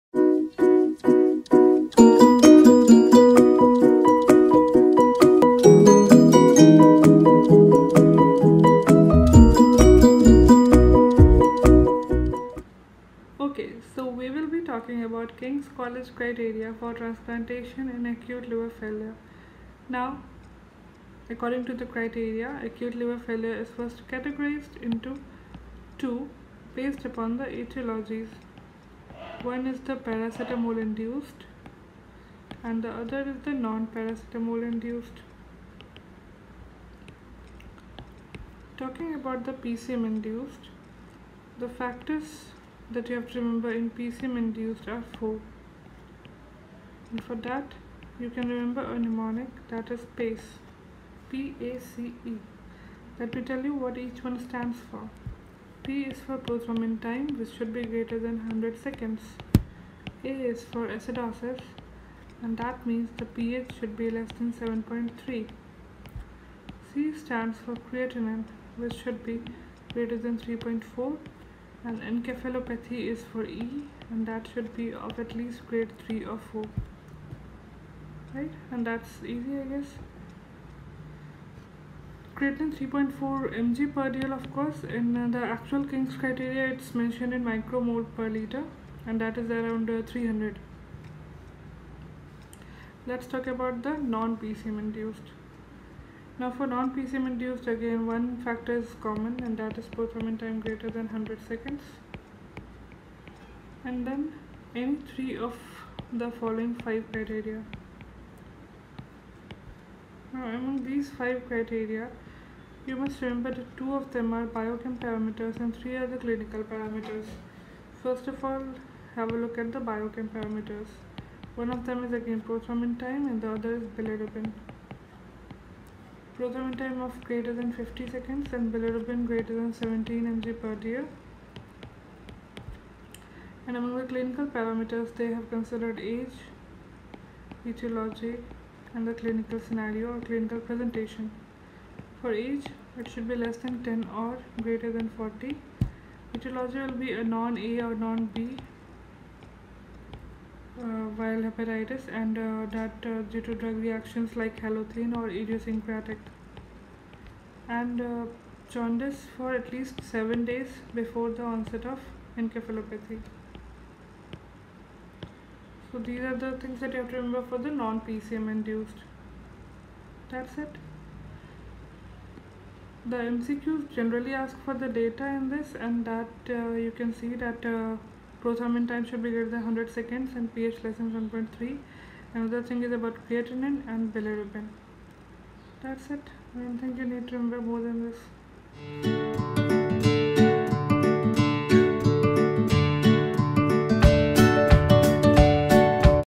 All you need to know about-Kings college criteria for transplantation in acute liver failure
Another video..with an attempt to make things easier to learn..please give your feedback in the comments section ..
my other videos you might be interested in